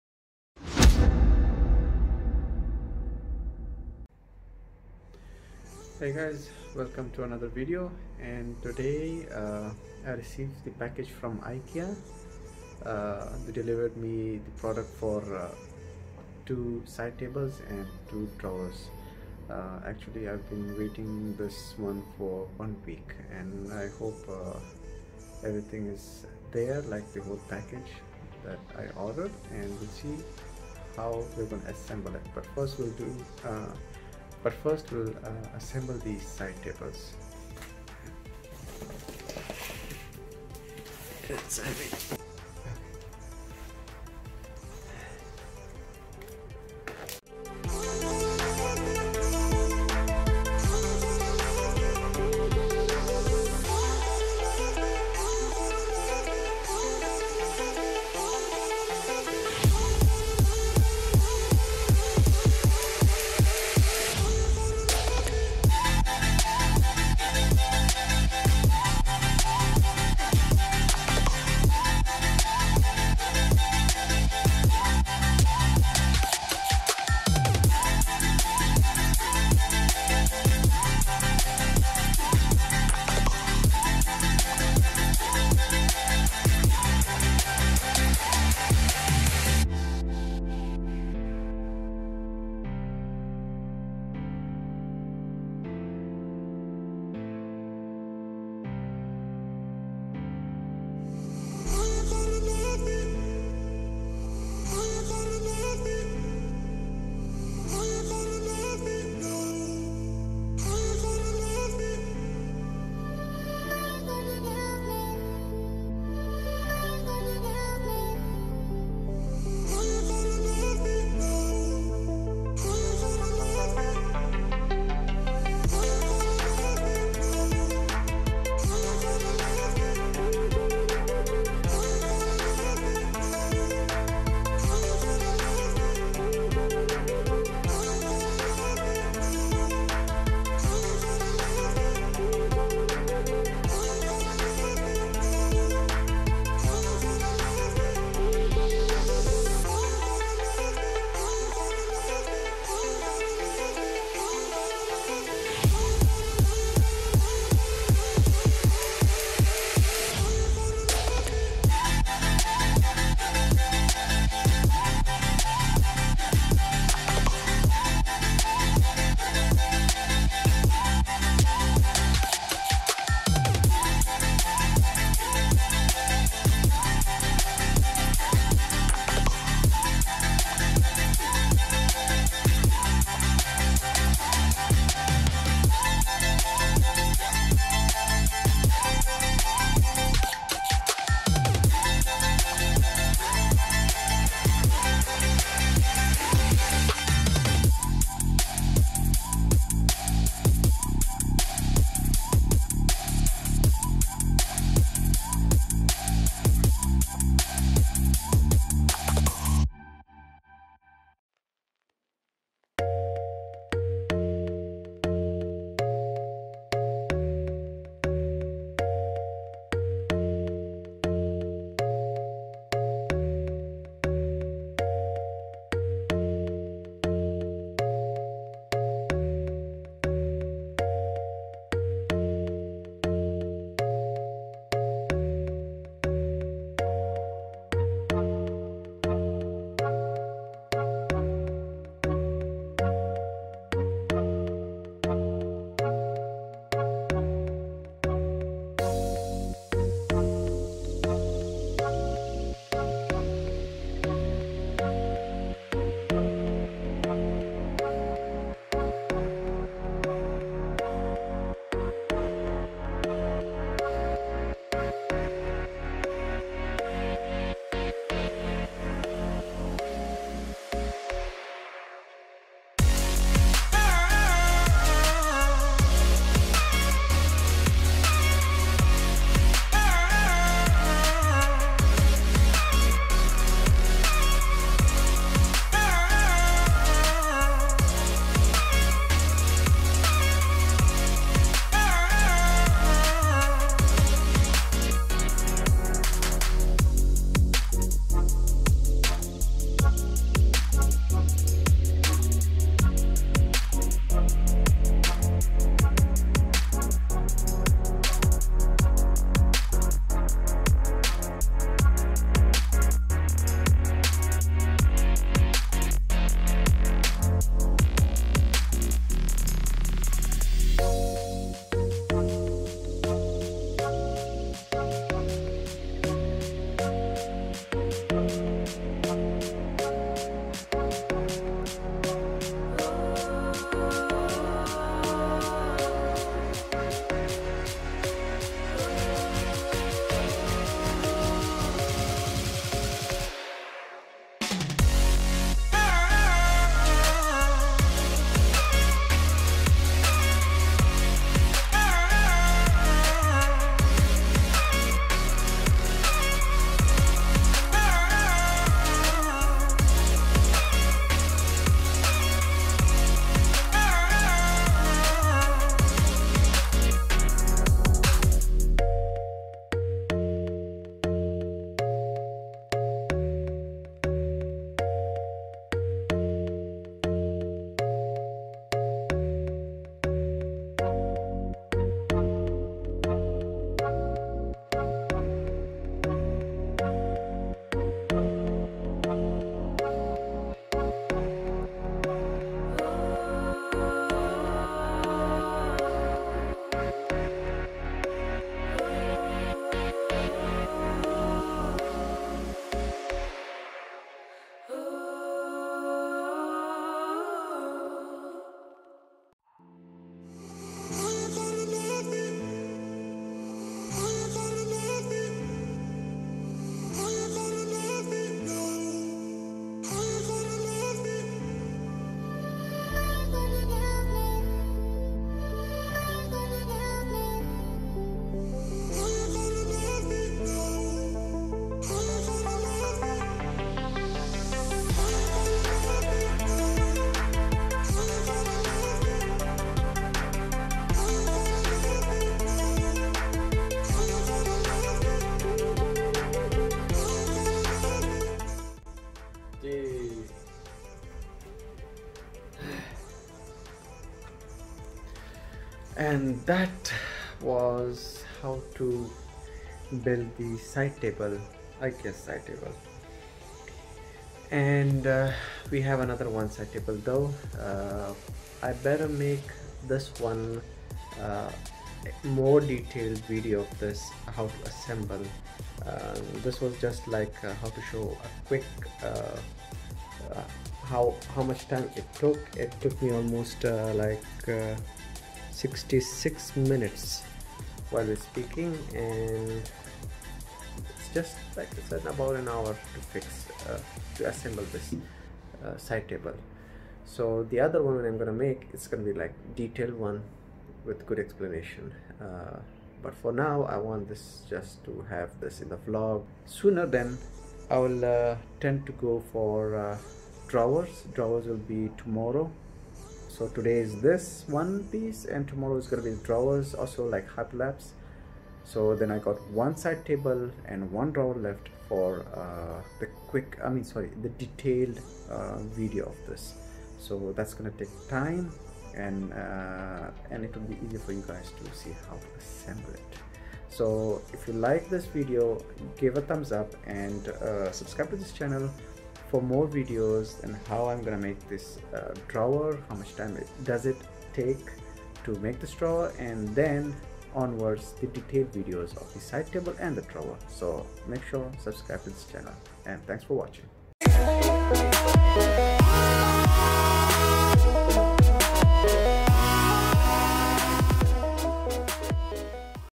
Assembling IKEA side table
Yo guys, purchased my first NEW IKEA products. Happy to assemble them. Hope a detailed building/assembly guide soon for you guys.